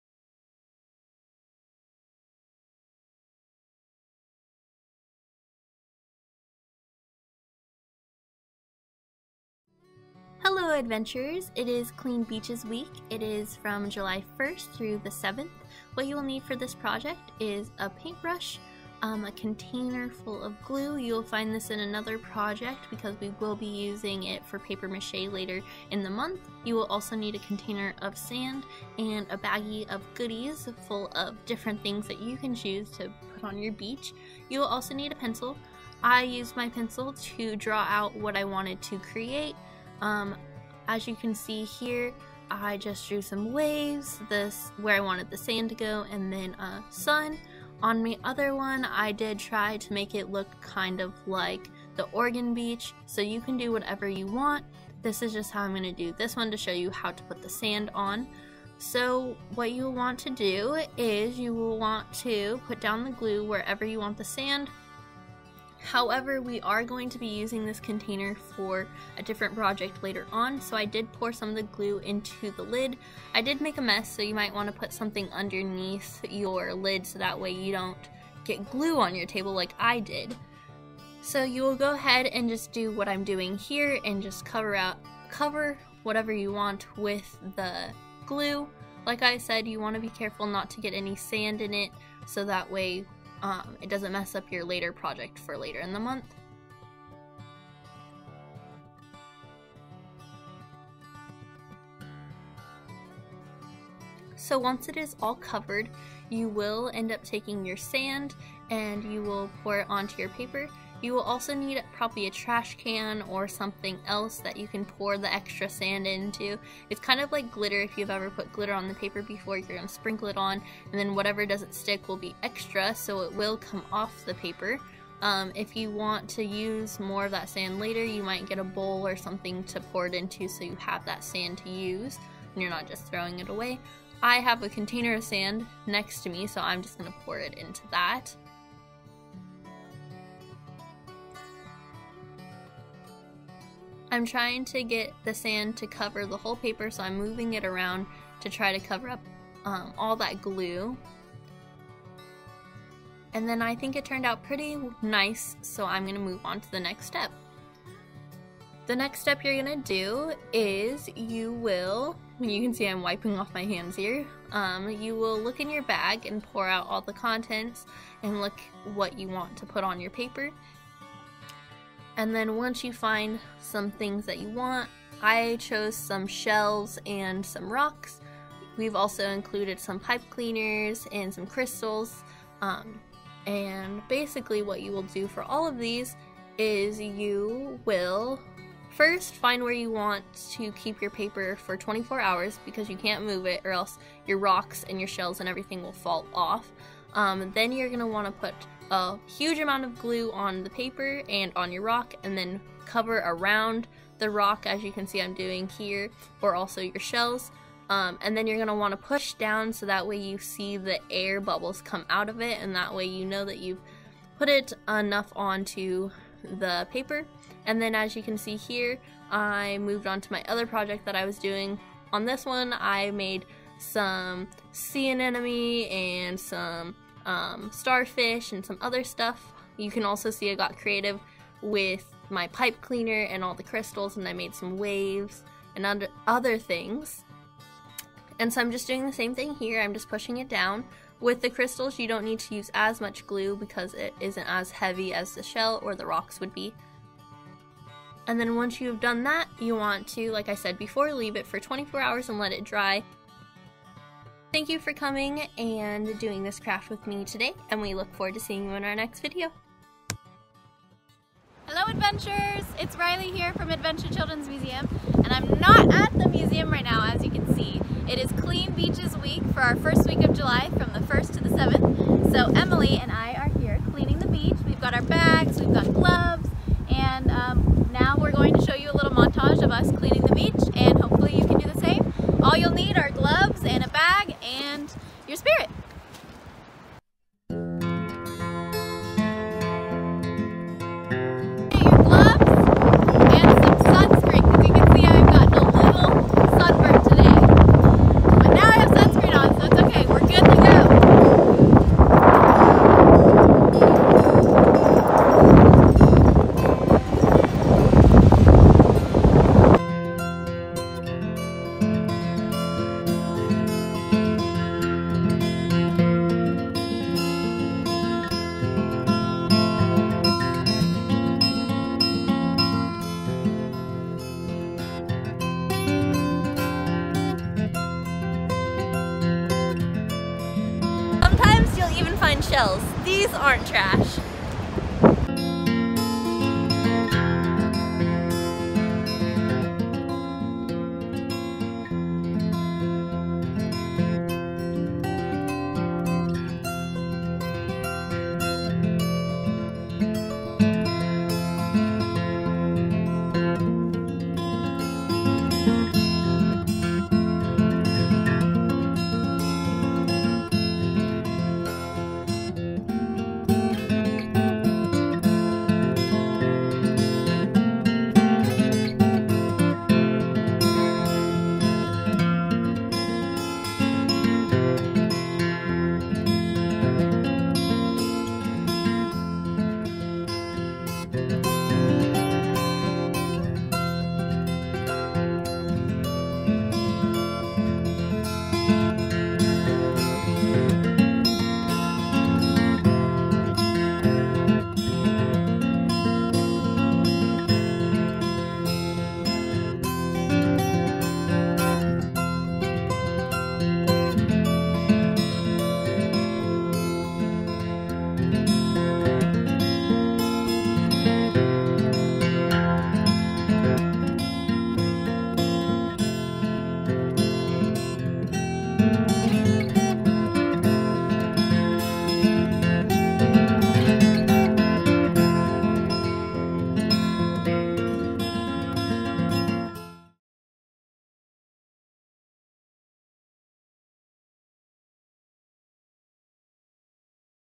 Adventure! Quick Crafts: Treasure Beach Art for Clean Beaches Week!
In this video, created for Clean Beaches Week, Rylie and Emily will show you how to create  3-D Treasure Beach Art, using beads, jewels, sand, and glue. And then they'll take you along on a beach clean-up! Enjoy Clean Beaches Week, Adventurers!